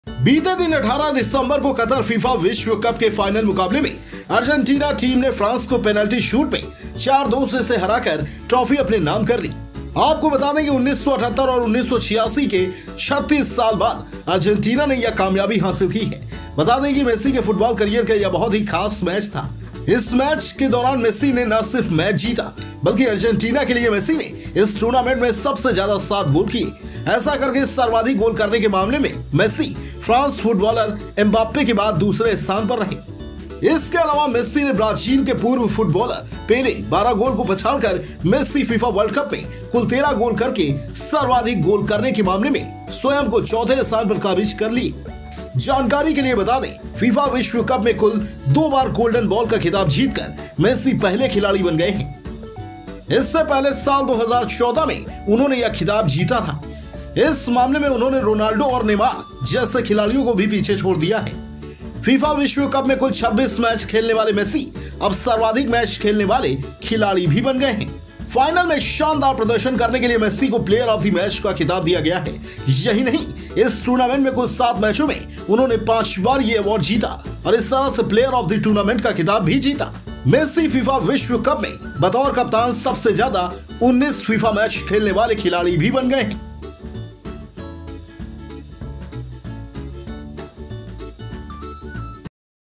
Lionel Messi ने FIFA World Cup 2022 में दर्ज किये 5 बड़े रिकॉर्ड
1978 और 1986 के 36 साल बाद अर्जेंटीना ने यह कामयाबी हासिल की है. वहीं इस इस मैच की सबसे खास बात यह रही कि मेसी के फुटबॉल करियर का यह आखिरी अंतर्राष्ट्रीय मैच था. ऐसे में इस मैच में मिली जीत ऐतिहासिक जीत मानी जा रही है. इस मैच के दौरान मेसी ने न सिर्फ मैच जीता, बल्कि अर्जेंटीना के लिए मेसी ने इस टूर्नामेंट में सबसे ज्यादा 7 गोल किए.